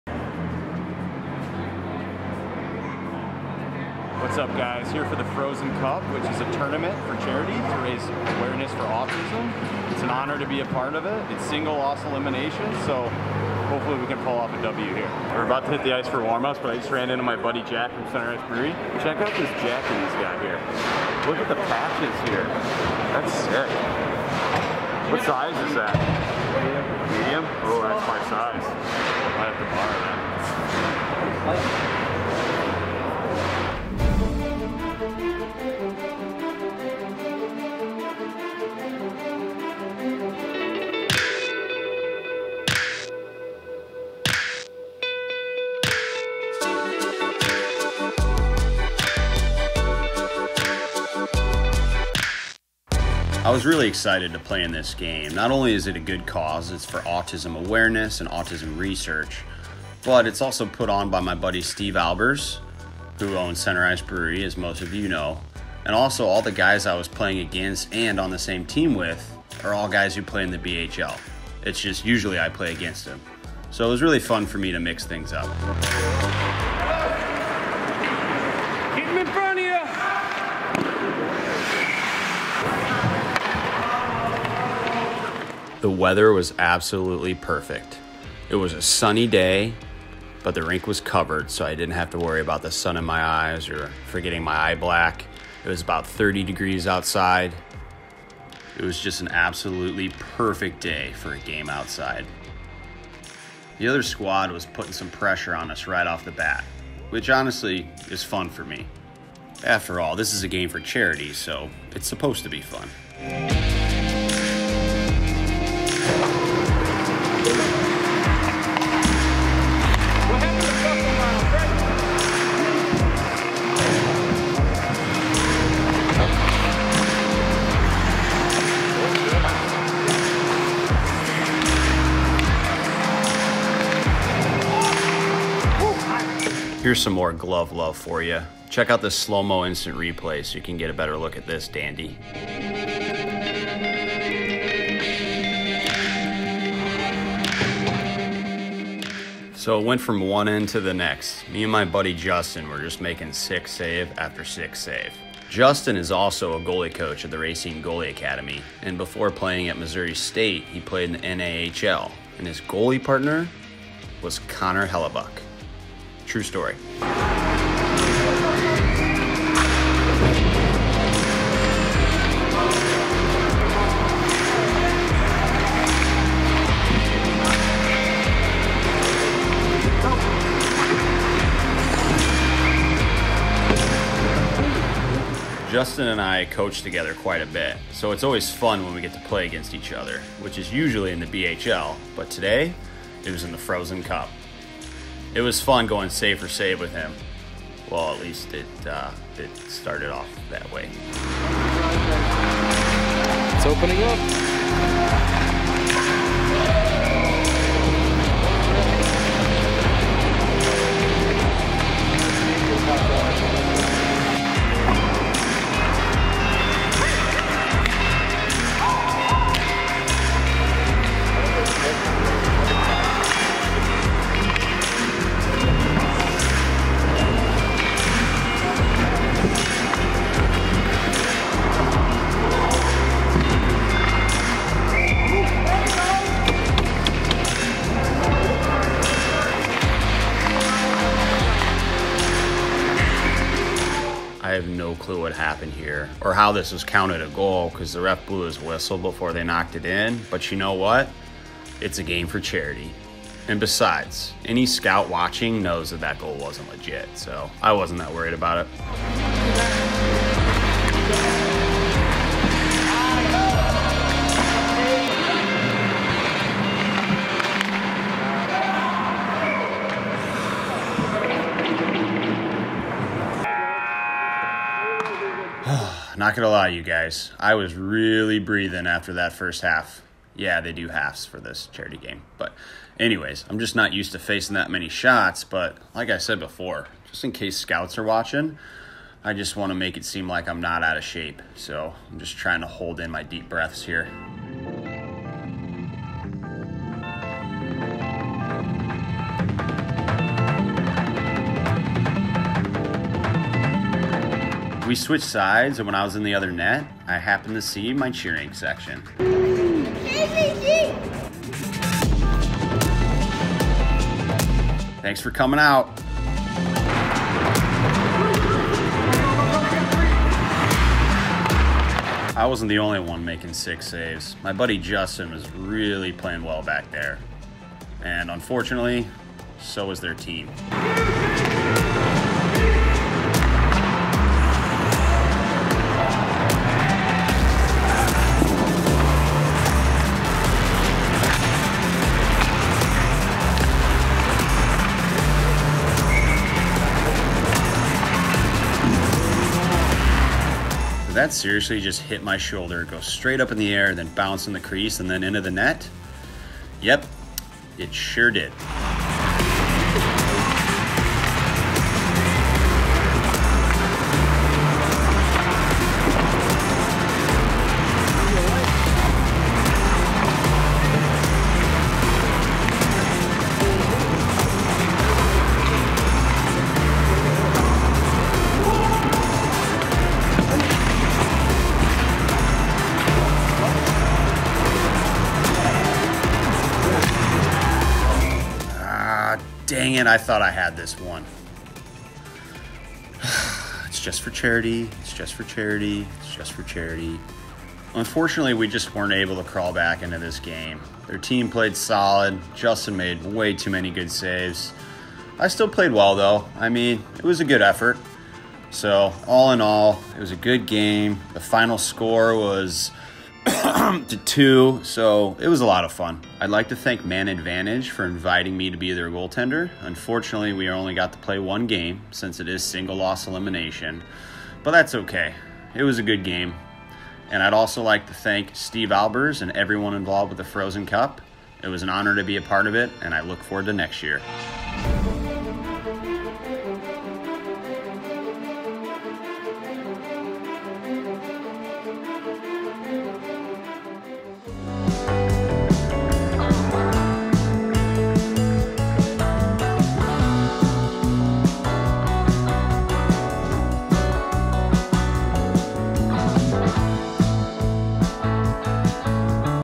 Frozen Cup 2020 | Getting Lit Up For A Good Cause… Again.
Kane Van Gate plays in the 2020 Frozen Cup in St. Louis, MO on the outdoor rink at the brand new Centene Community Ice Center.

Special thanks to the Frozen Cup, Center Ice Brewery, Steve Albers, Jack, and Team Man Advantage.

@KaneVanGate38 (Twitter)
@KaneVanGate38 (Instagram)